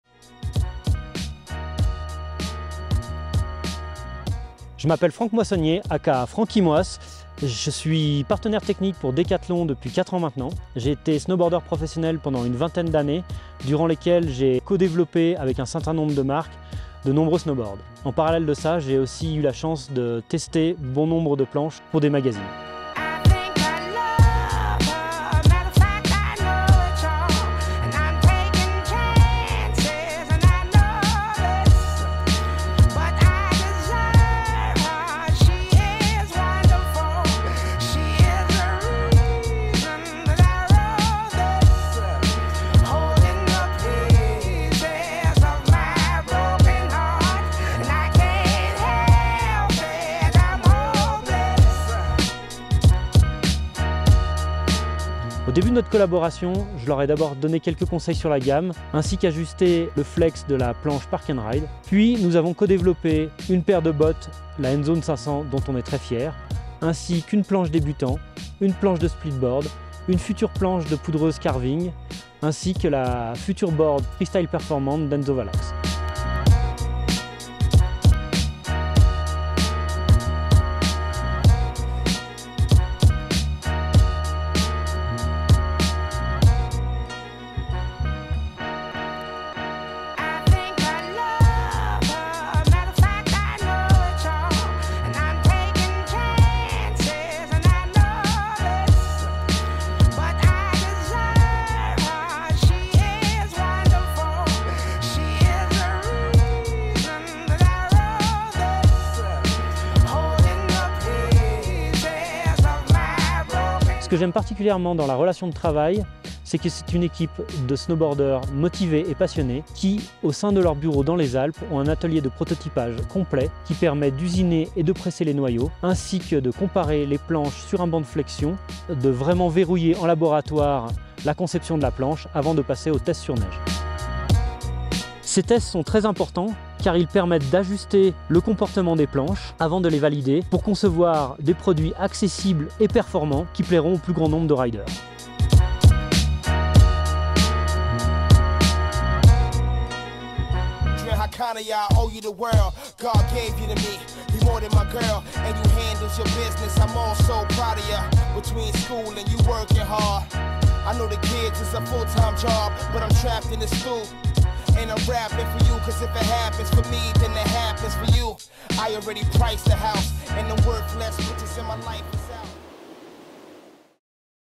Designing snowboard with FRANKY MOISS, our technical partner !
Follow our technical partner,  Franky Moiss, in our R&D and design center located right at
 the bottom of the Mont-Blanc...